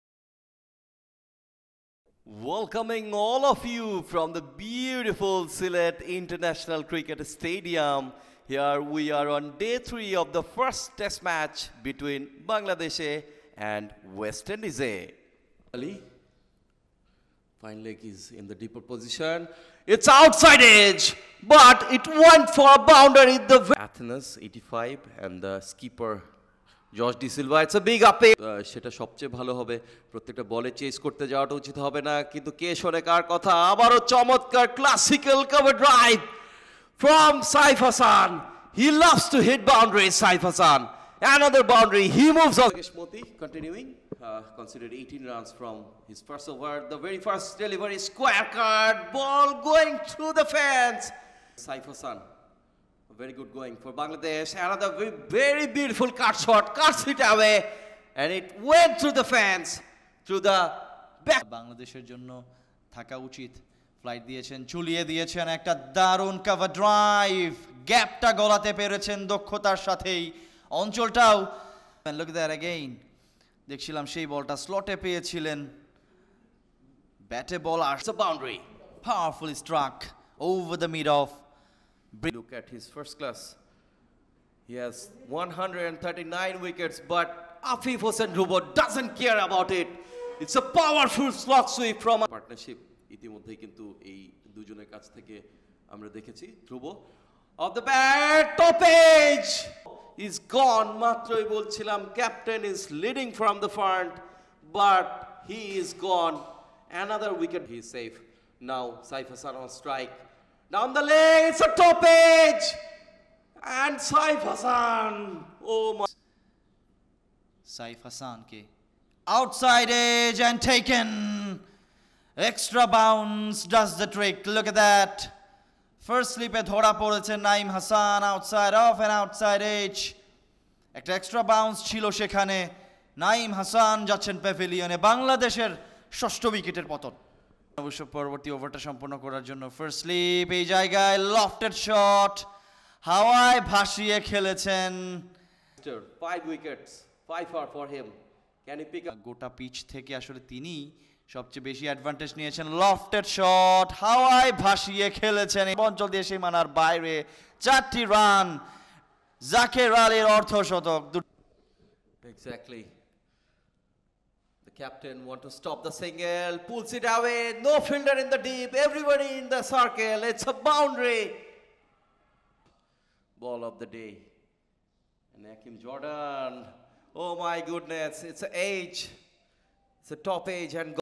Highlights | Day 03 | Bangladesh A vs West Indies A | 1st Four-Day Match
West Indies A tour of Bangladesh 2023
Bangladesh A vs West Indies A | 1st Four-Day Match | Day 03 | Highlights